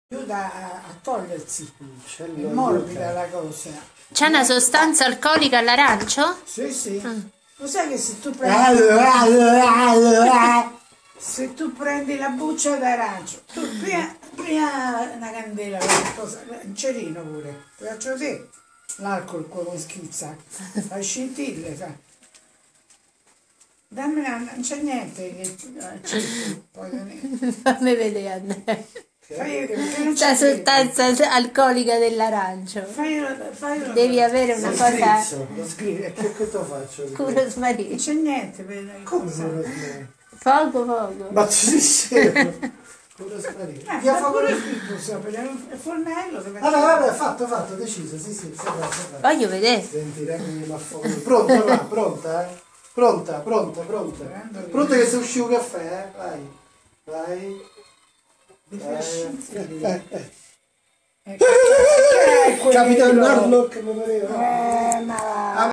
"Spirito di Vino " ossia Pasqua 2015 con la Regina Madre
Complice il Rosso Piceno 'Terre di Giobbe' di Fiorano sua Altezza la Pasqua Mesta ci racconta della profezia dell'arancio